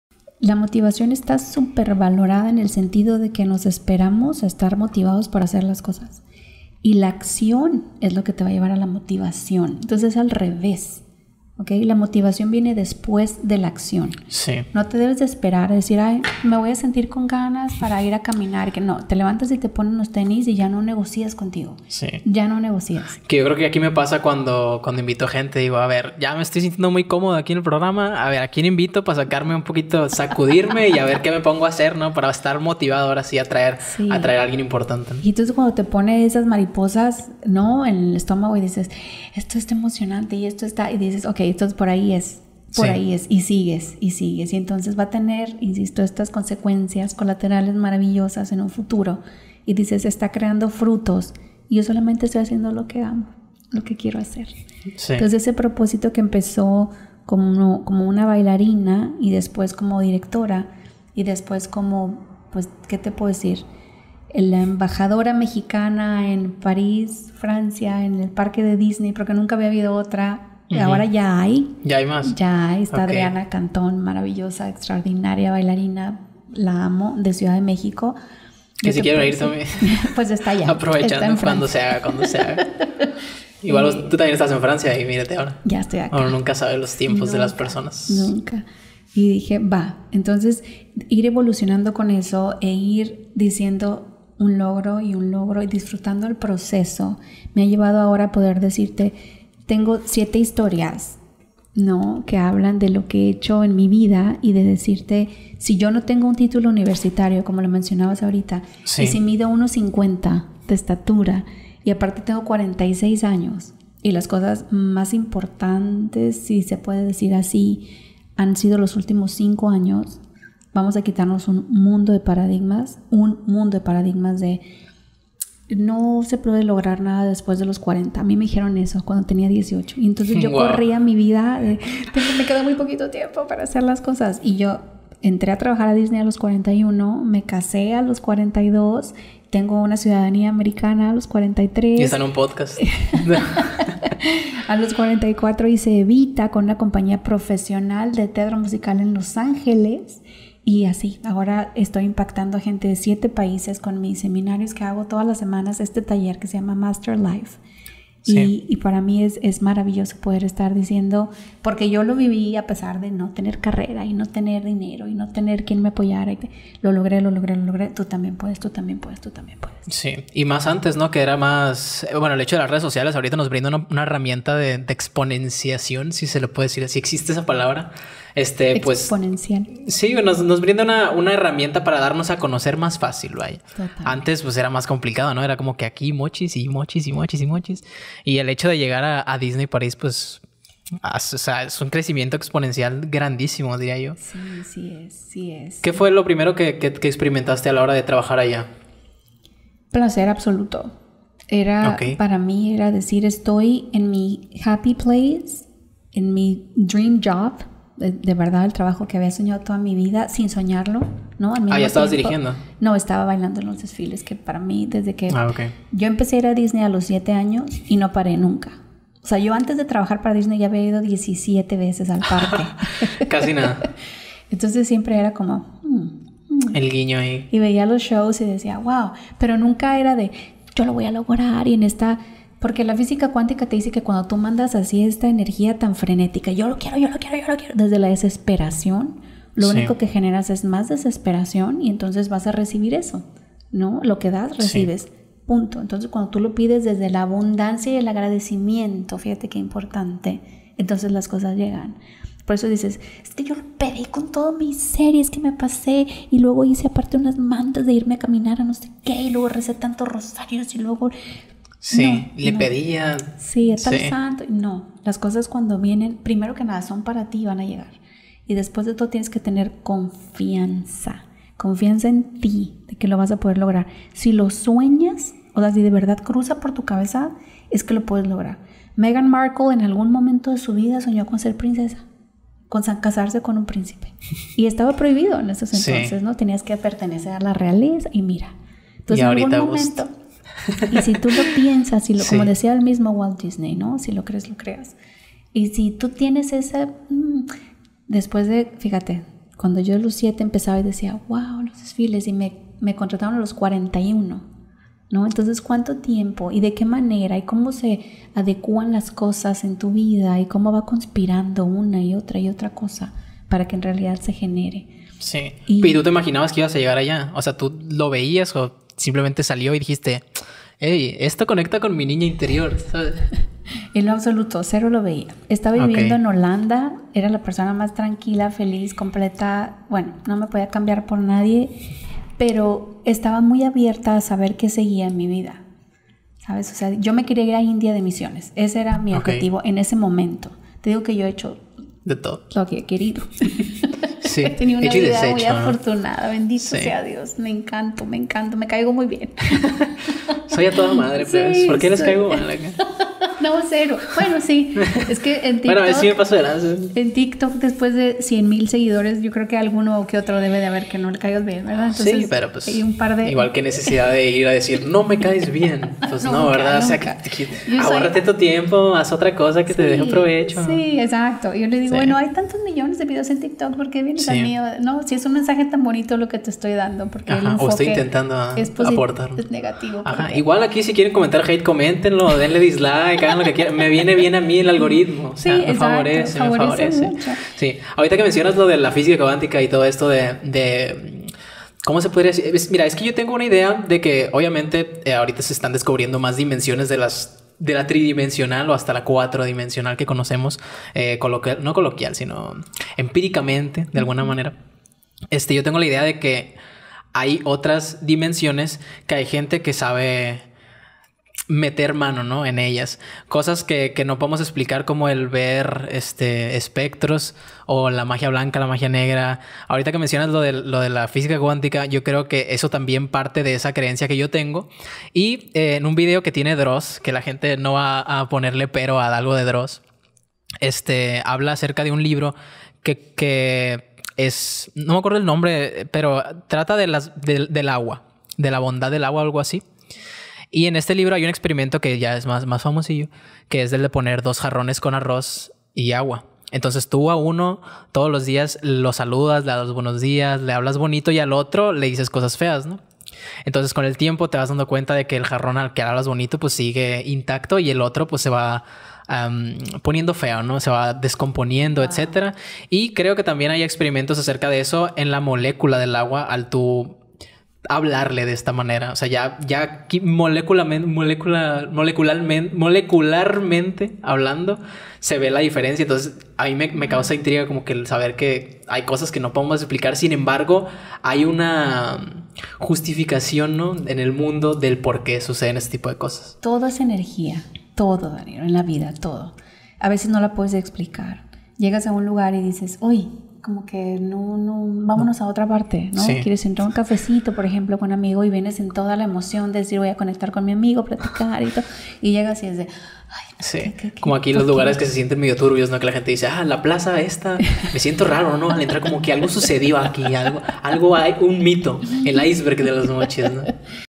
MANIFESTAR Y VIBRAR ALTO CON PATY SILVA II ESTE PODCAST YA EXISTE (O NO) II
Visualizar, manifestar y pensar positivo como clave de tus logros.